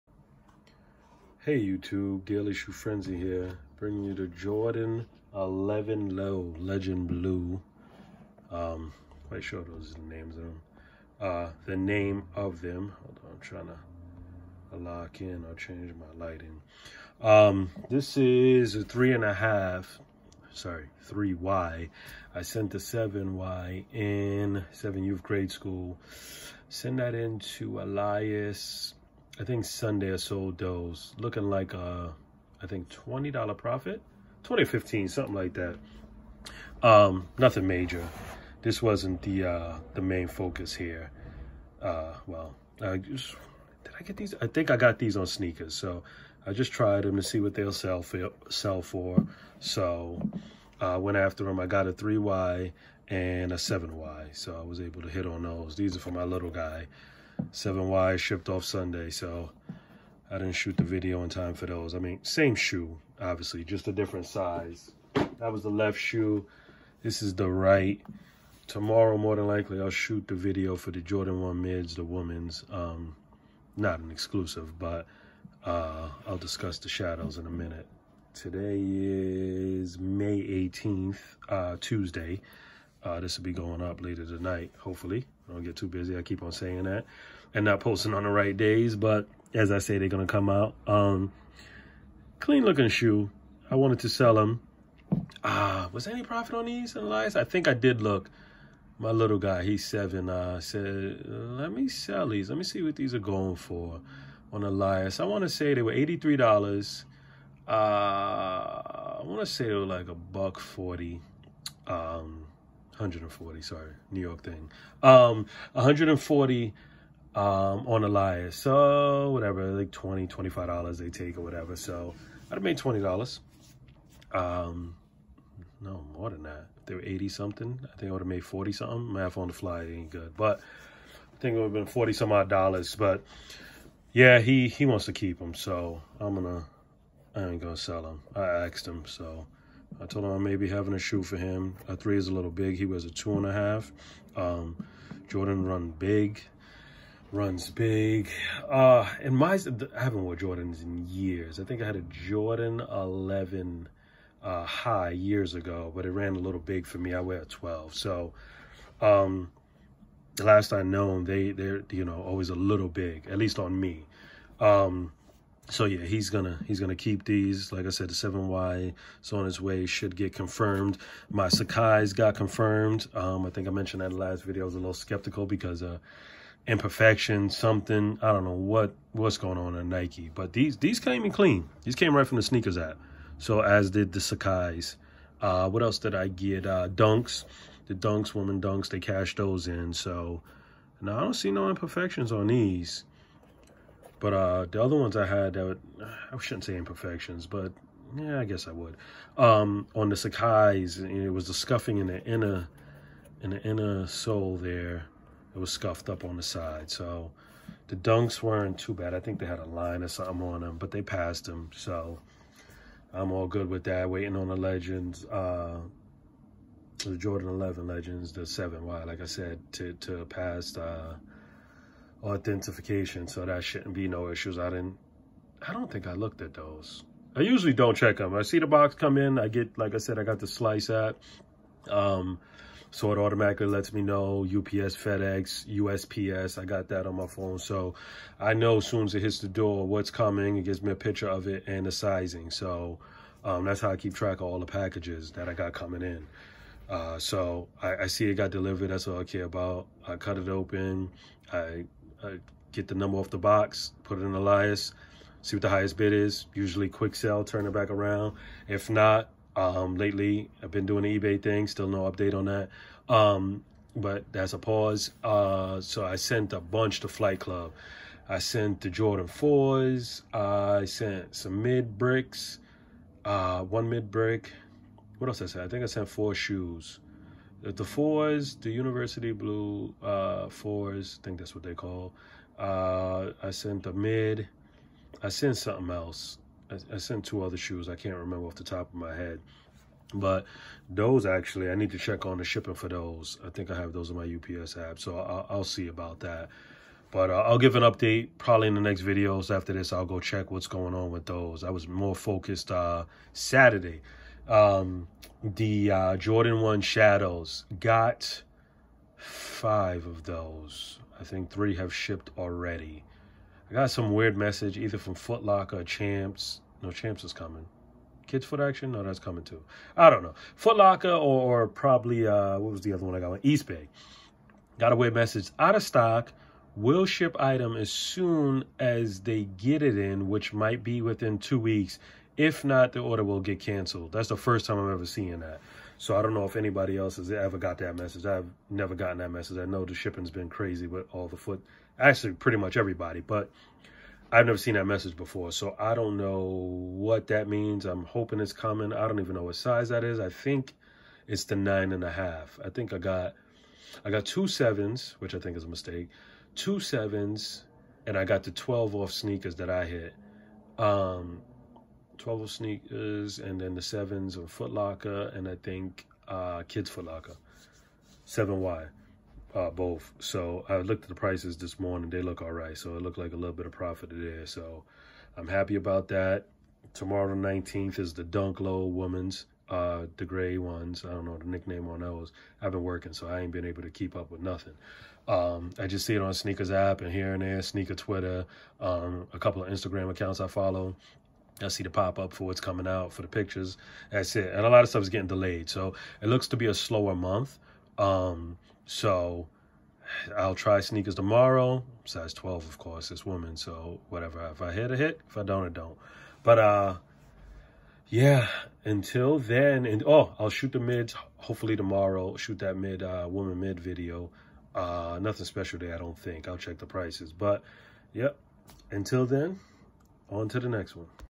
JORDAN 11 LENGEND BLUE
Story time with the Jordan 11 low  lengend blue and my current resale items